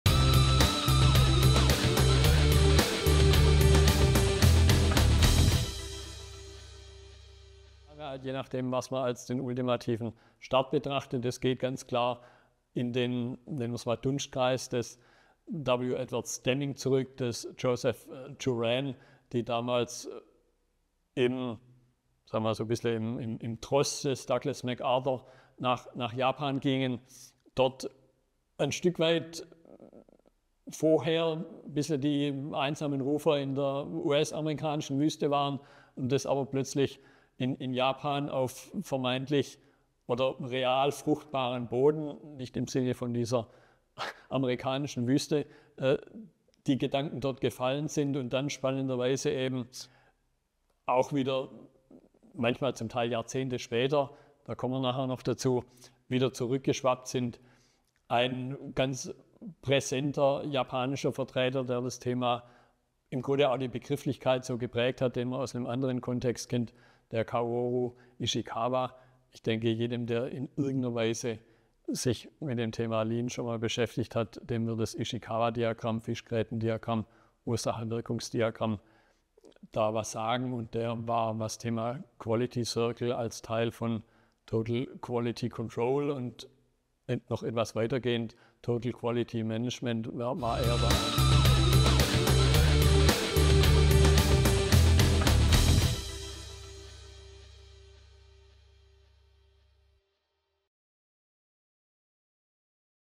Grundprinzipien der Quality Circles
Dieser Teaser aus dem Online-Kurs QualityCircle beschreibt die Grundprinzipien und Charakteristiken von Quality Circles, die Voraussetzungen und mögliche Resultate. Dazu gehört auch die praktische Vorgehensweise für die Aktivitäten in den Quality Circles, ebenso wie die Wechselwirkungen in andere Bereiche des Unternehmens und seiner Geschäftsprozesse. Das Verständnis der Grundprinzipien ist ein entscheidender Erfolgsfaktor für den Einstieg in Qualitätszirkel, wie auch der Pflege und die kontinuierliche Weiterführung.